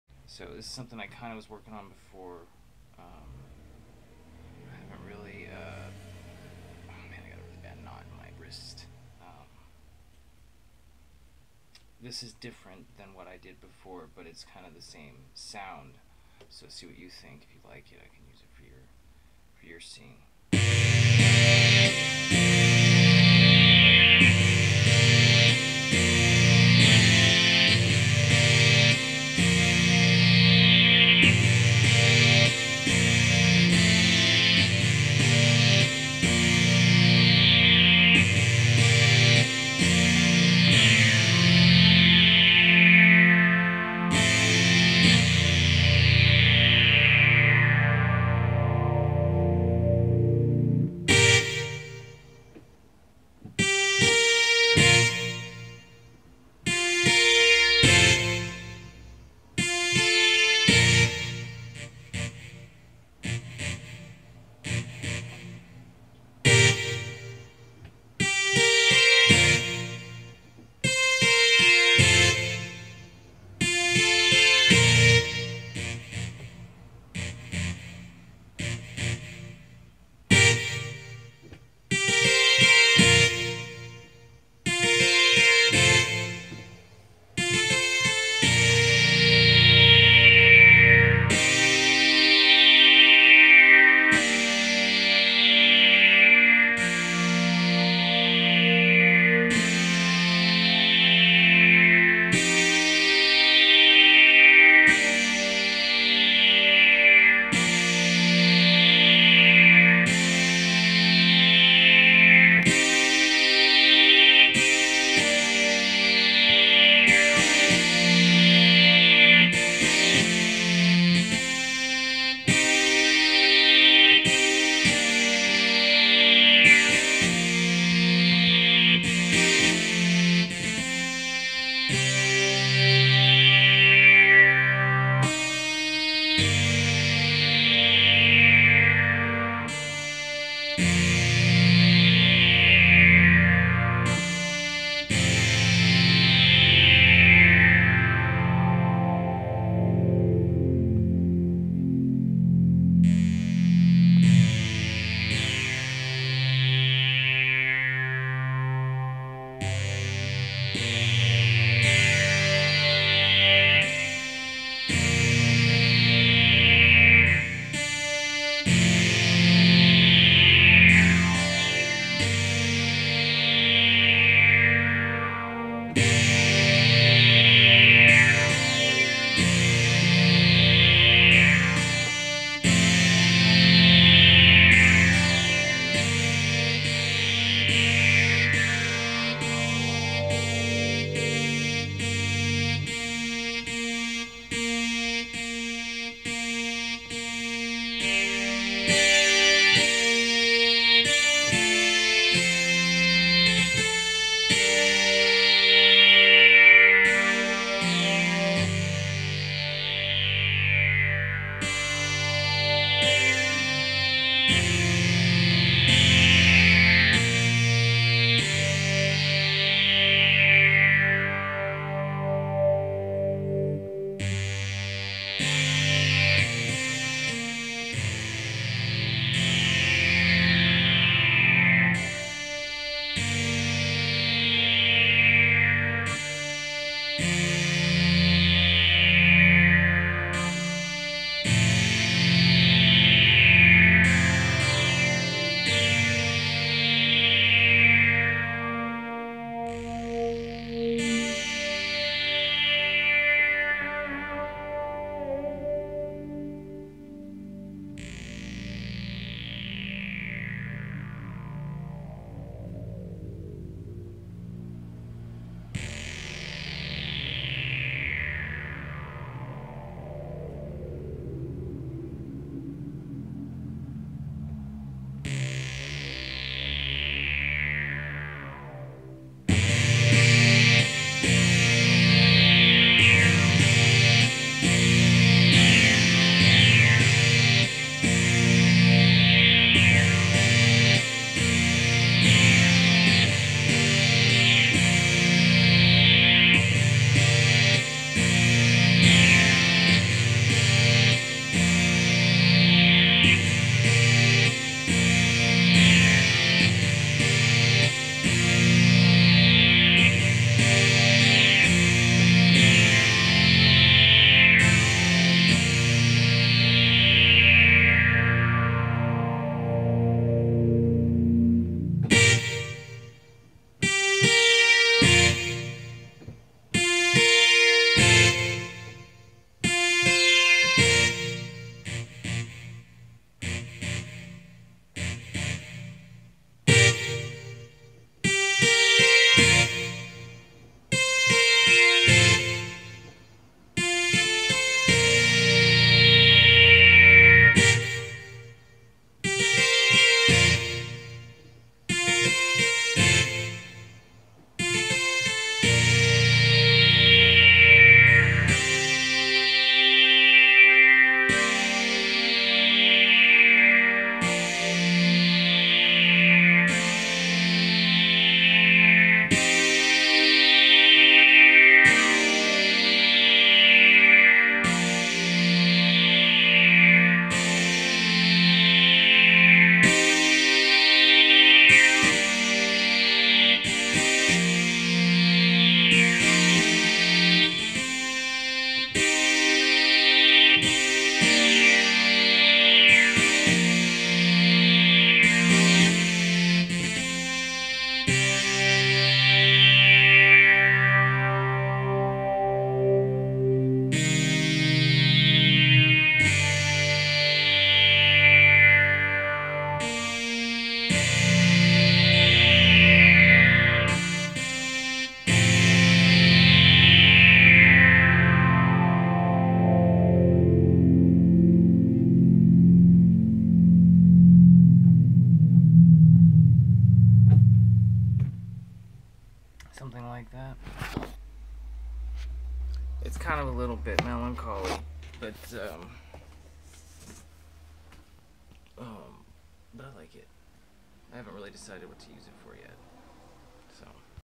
An Idea for Kylie, Brittany, or Jade
I am not sure whom to use this for. This may be more of a song about my state of mind, but I can feel each one of them in it. I sense Hope and Melancholy within it. I  may end up using it as an Atlas Anthem, if none of the models like it.